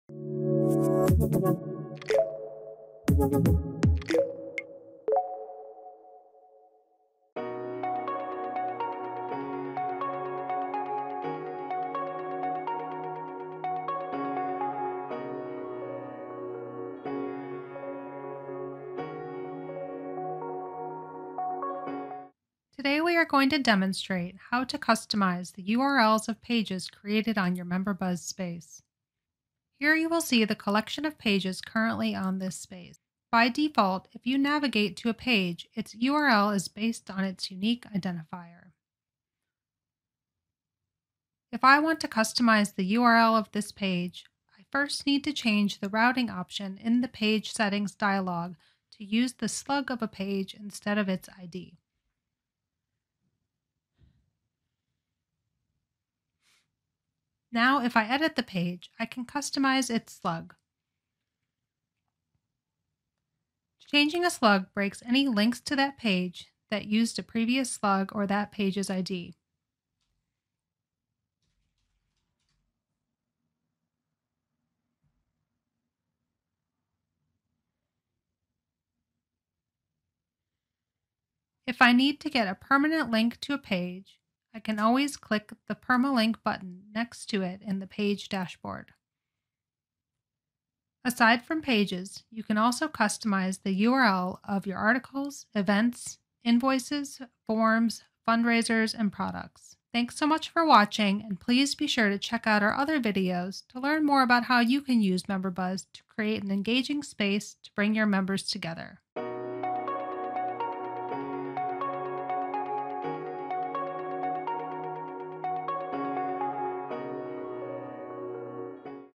Customizing Page URLs | Member.buzz
Learn how to customize the URL of your Articles, Events, Pages, and more in your Member.buzz Space.